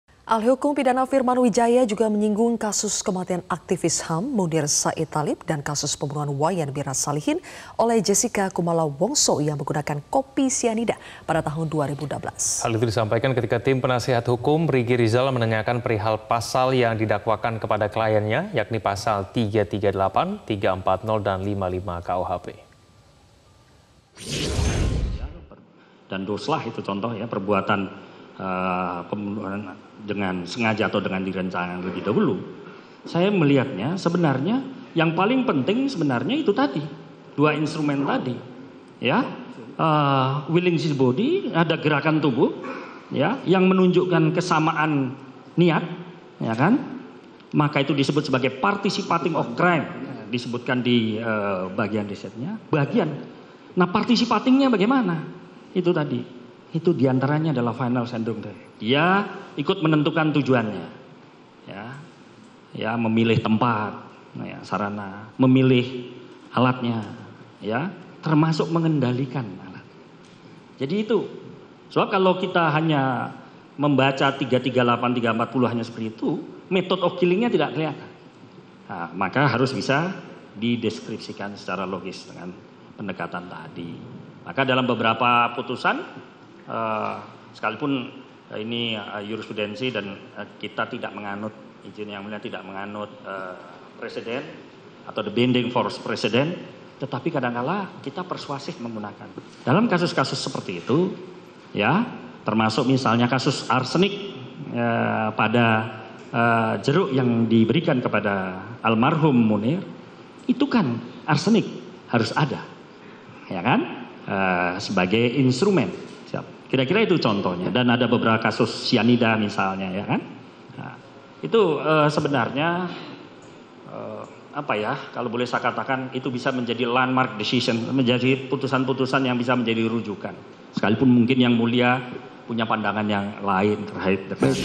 Saksi Ahli Singgung Kasus Munir Dan Kopi Sianida
MetroTV,
Ahli hukum pidana Firman Wijaya menyinggung kasus kematian aktivis HAM, Munir Said Talib, dan kasus pembunuhan Wayan Mira Salihin oleh Jessica Kumala Wongso yang menggunakan kopi sianida pada 2016.

Hal tersebut disampaikan ketika tim penasihat hukum Ricky Rizal menanyakan perihal pasal yang didakwakan kepada kliennya, yakni Pasal 338, 340, dan 55 KUHP.